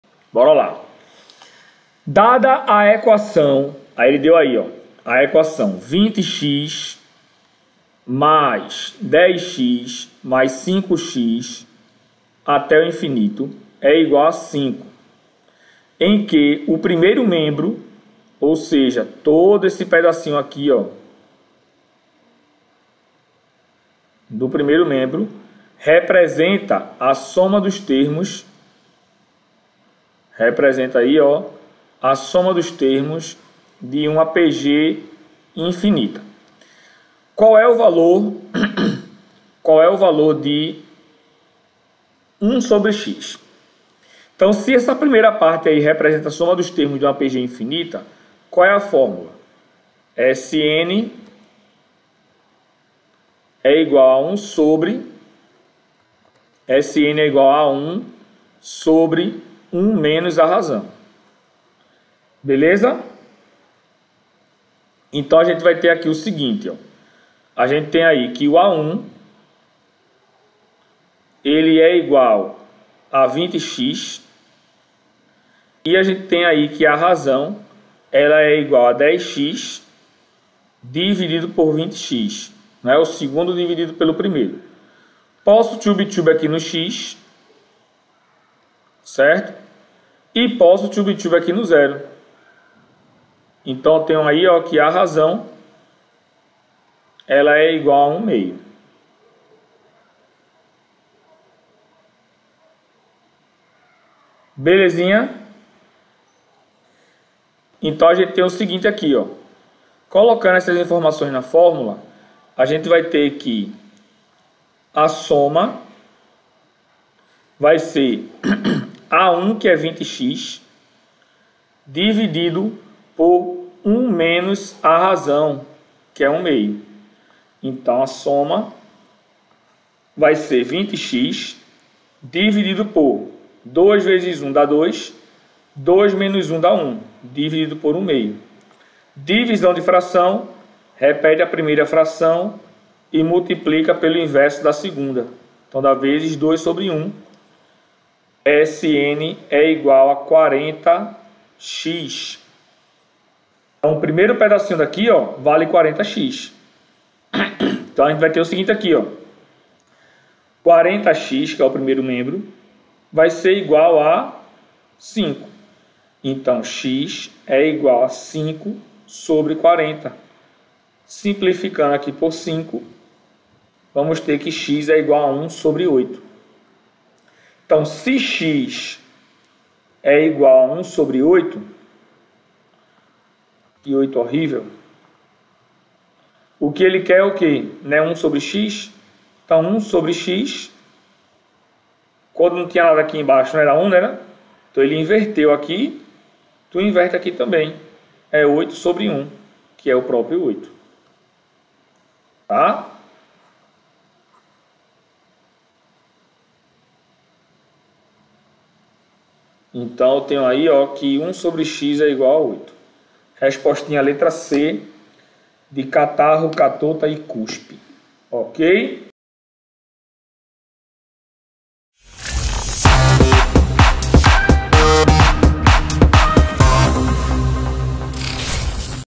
EEAR | PROGRESSÃO GEOMÉTRICA
Link do DESBLOQUEANDO A MATEMÁTICA

Fala turma segue a solução dessa questão!!!

04. Dada a equação 20x + 10x + 5x + … = 5, em que o primeiro membro representa a soma dos termos de uma progressão geométrica infinita, o valor de 1/x é

A) 12
B) 10
C) 8
D) 5​
E) 3

Qualquer dúvida estou à disposição!!!
SIMBORAAAAA